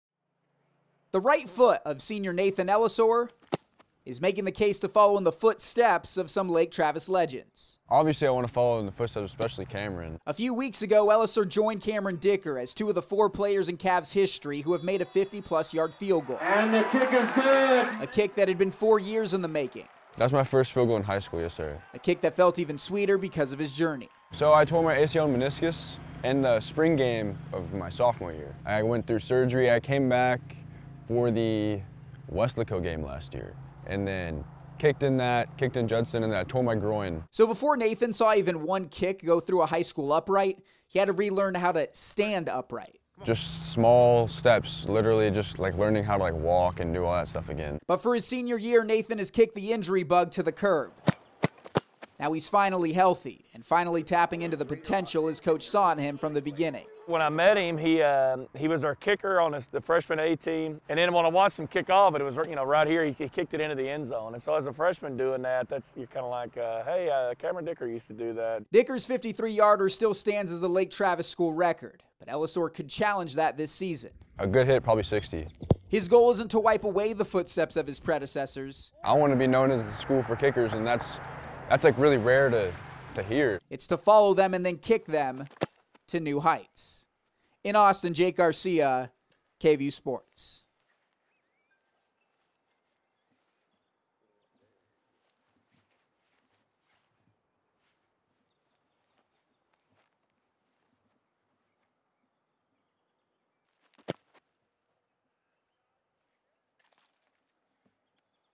Lake Travis kicker hits history-making field goal | KVUE
Lake Travis High School is known for producing quarterbacks, but there's another position where they thrive that you might not think of. KVUE's Jake Garcia explains.